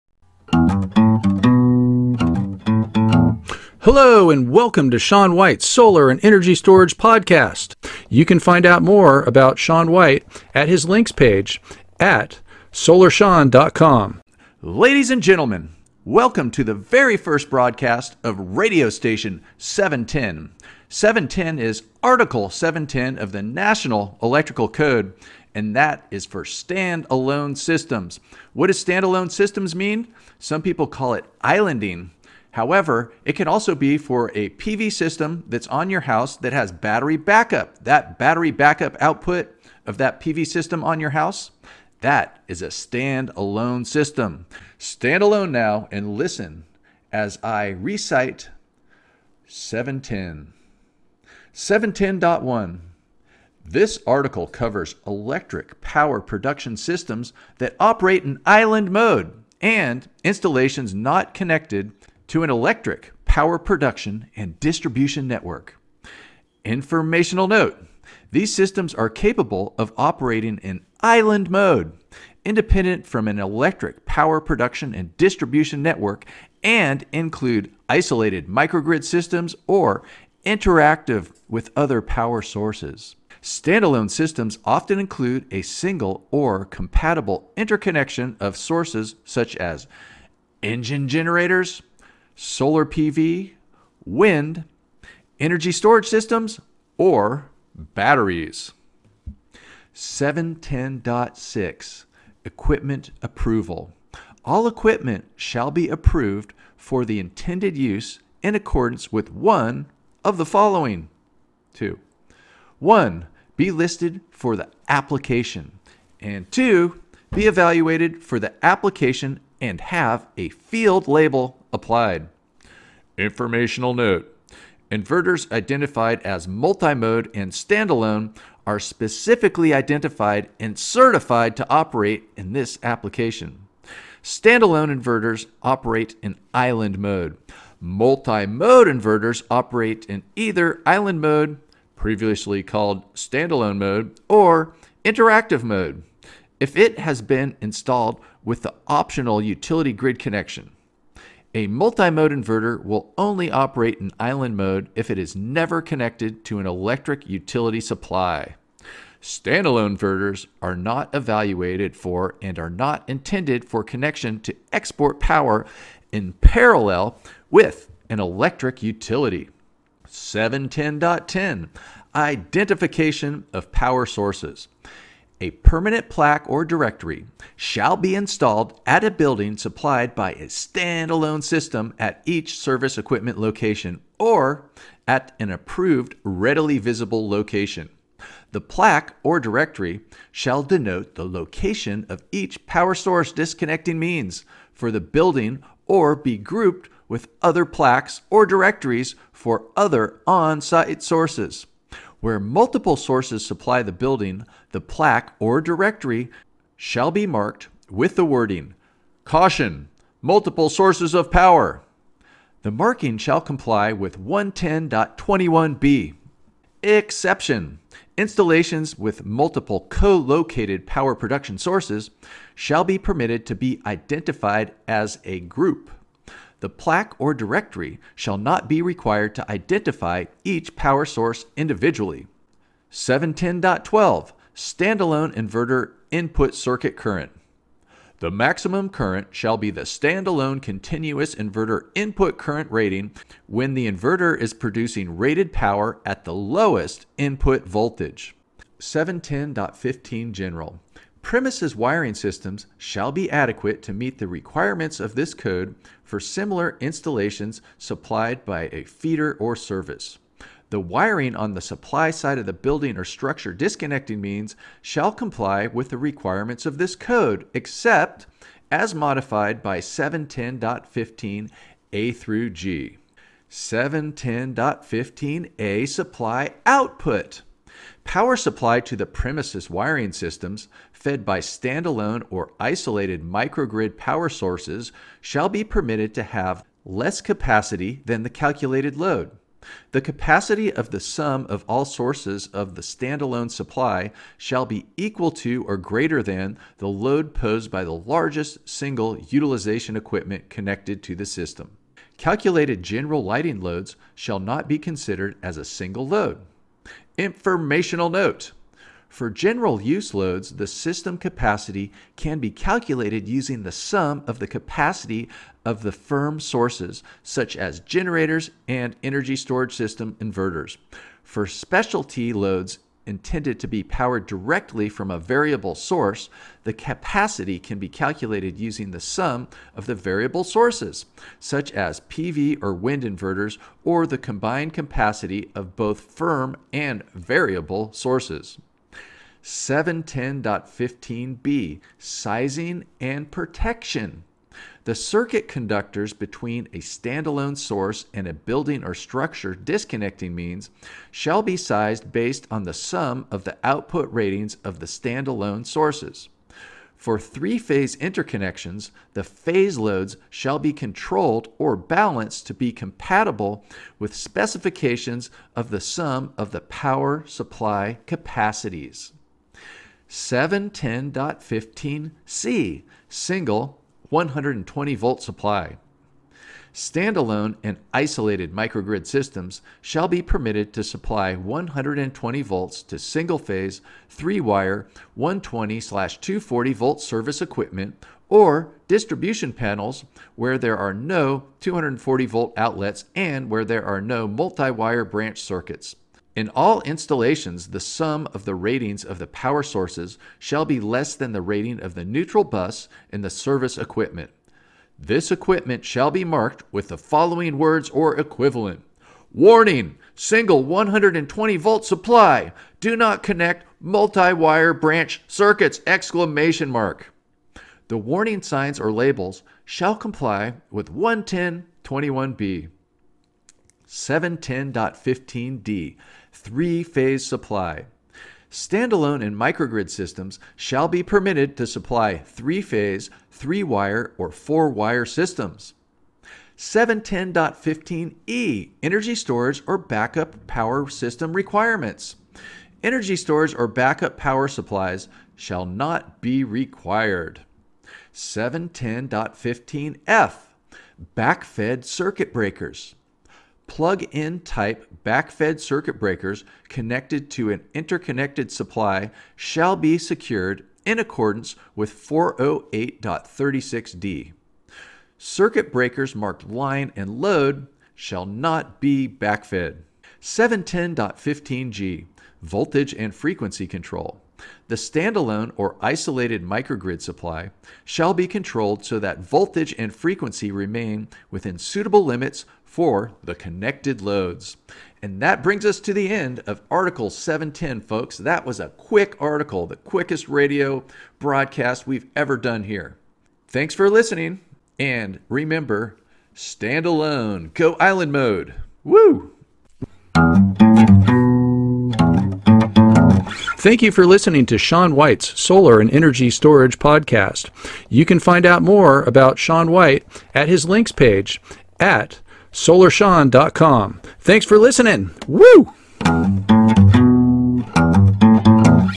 2020 NEC Radio 710 Stand-Alone Systems
NEC Article 710 Stand-Alone Systems is short and this reading will get you thinking about living off the grid or will get you ready for when the apocalypse comes and your prepper tendencies pay off!